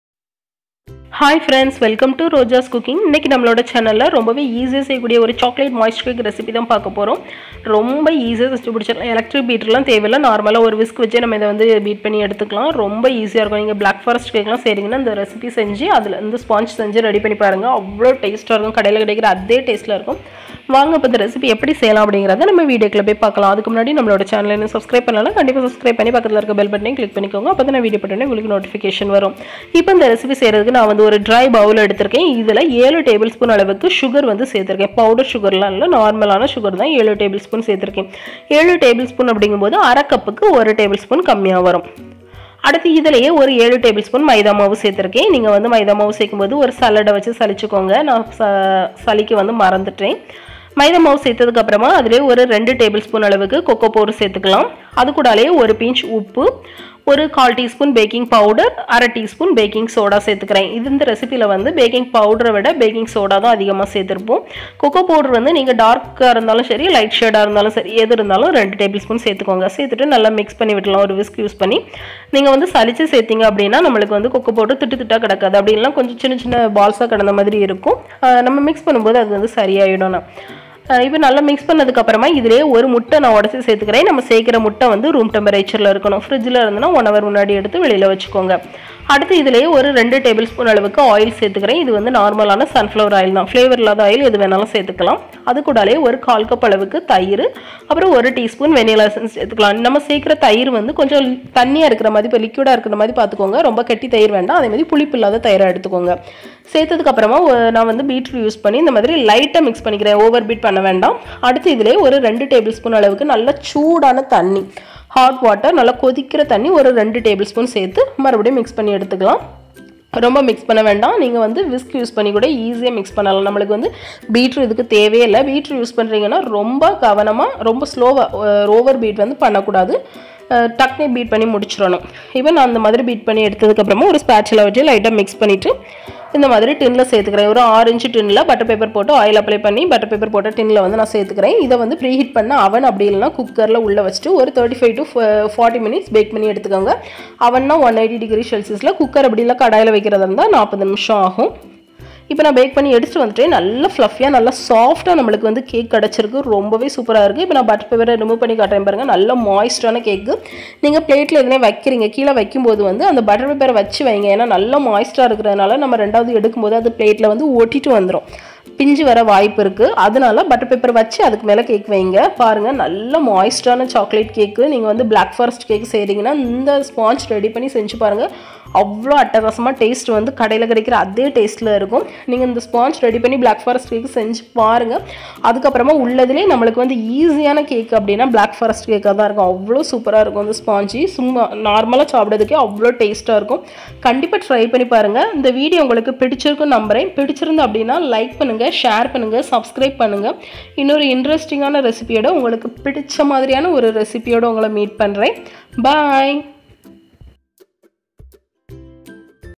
Perfect Chocolate Moist Cake | Black forest sponge recipe | Cake Recipes in Tamil | ROJAS COOKING 🌹
HI FRIENDS, WELCOME TO ROJAS COOKING 🌹

TODAY RECIPE IS Perfect Chocolate Moist Cake | Black forest sponge recipe | Cake Recipes in Tamil | ROJAS COOKING 🌹

INGREDIENTS
SUGAR : 7 TBSP
MAIDA : 7 TBSP
COCOA POWDER : 2 TBSP
SALT : 1 PINCH
BAKING POWDER : 1/4 TSP
BAKING SODA : 1/2 TSP
EGG : 1
OIL : 2 TBSP
CURD : 1/4 CUP
VANILLA ESSENCE : 1 TSP
HOT WATER : 2 TBSP

                          THANK U 😊